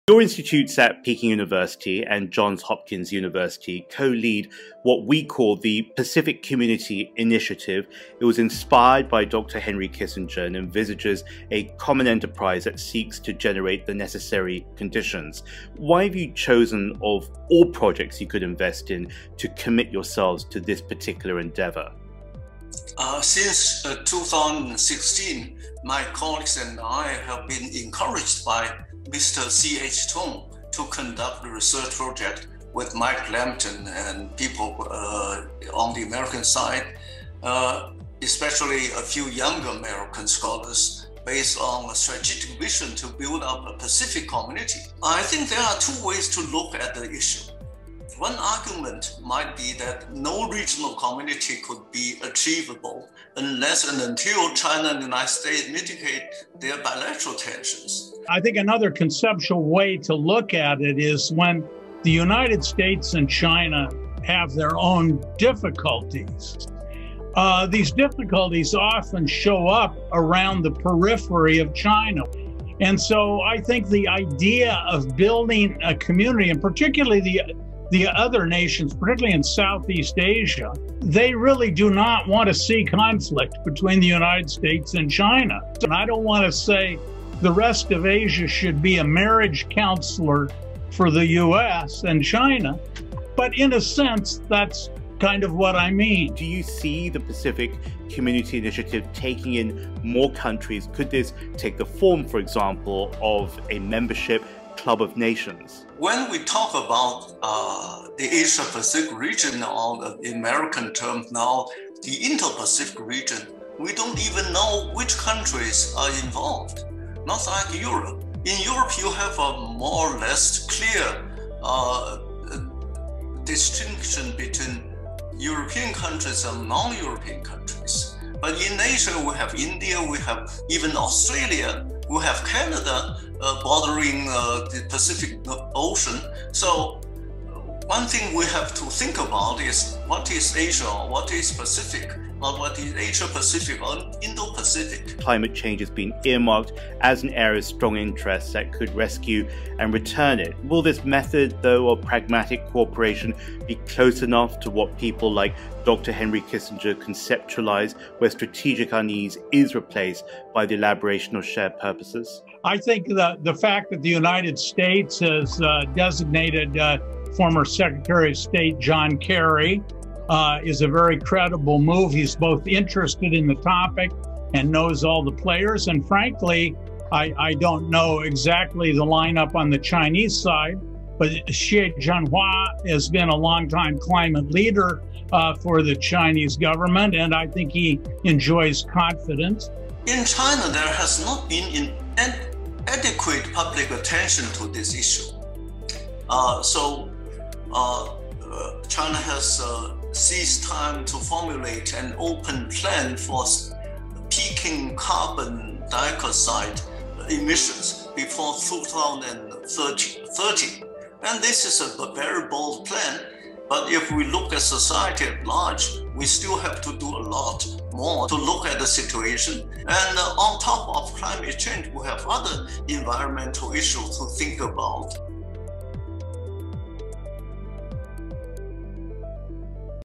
Asia as Marriage Counsellor? | Part 2
There’s a vision for a Pacific community that would safeguard the world, and that idea has inspired the work of a special initiative co-led by Professor Wang Jisi of Peking University and Professor David Lampton of Johns Hopkins University.

They explore the role of other countries impacted by the shifting U.S.-China dynamic. Are other Asian countries getting caught in the rivalry between  two global powers — or are they playing the role of marriage counsellor to both? Moderated by James Chau.